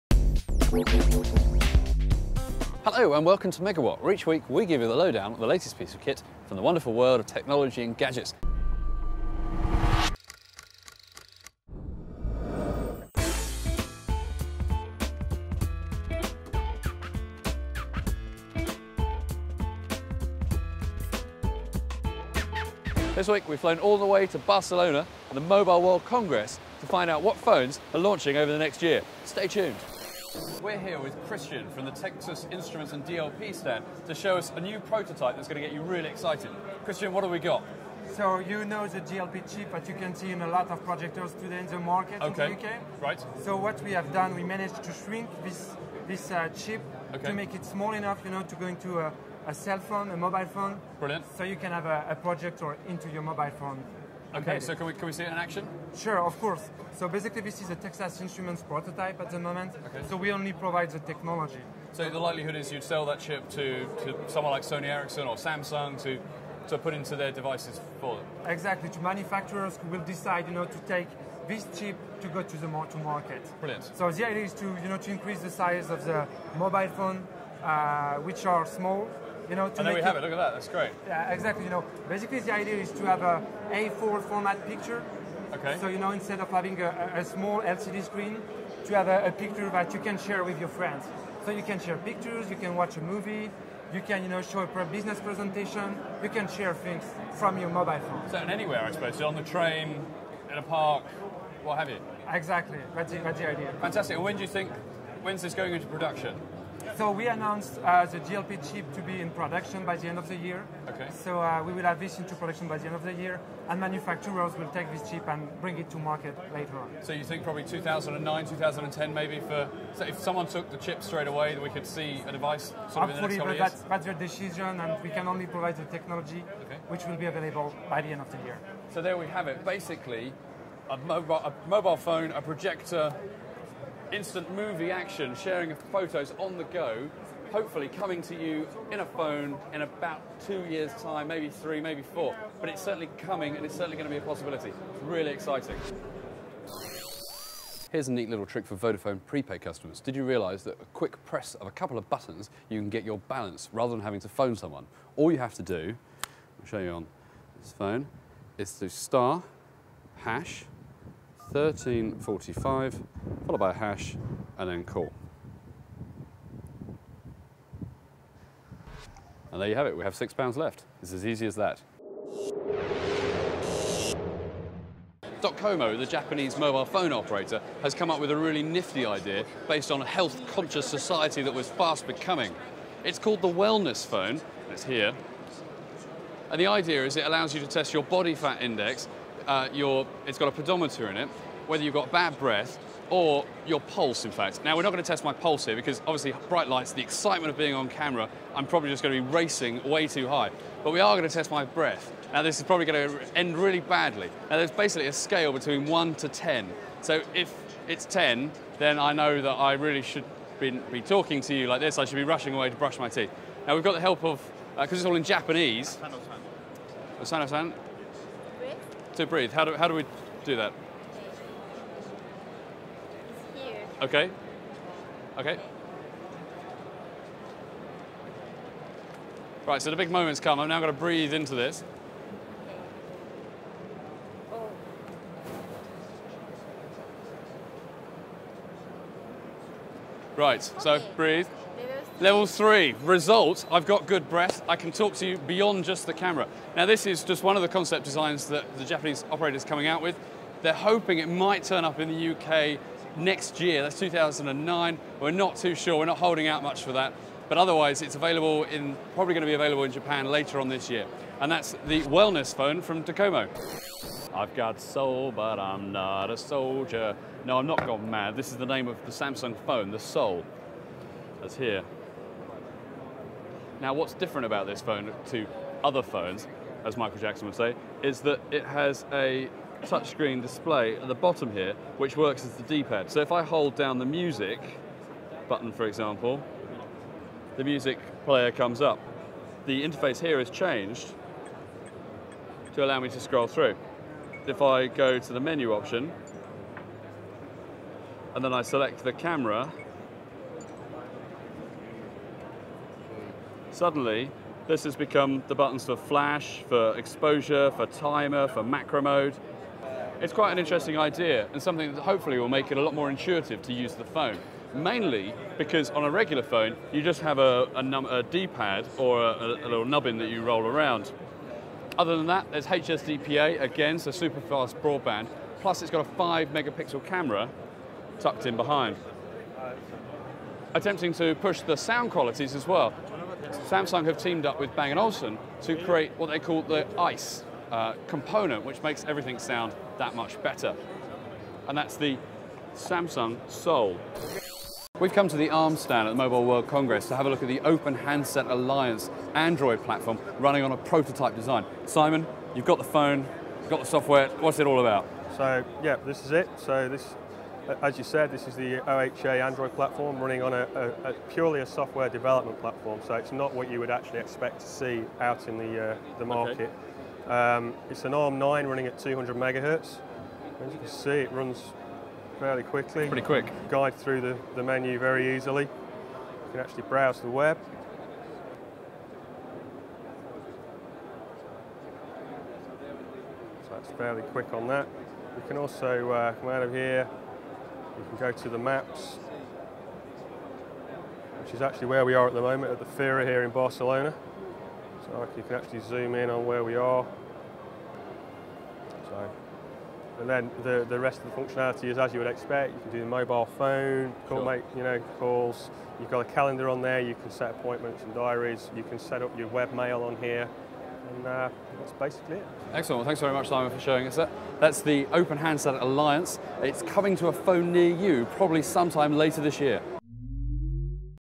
Mobile World Congress 2008: First Look Reviews (Part01of02)
First looks at the Samsung Soul.  Plus hands on with Google Android, the bizarre Wellness phone concept repleate with bad breath tester and Pico phone projector...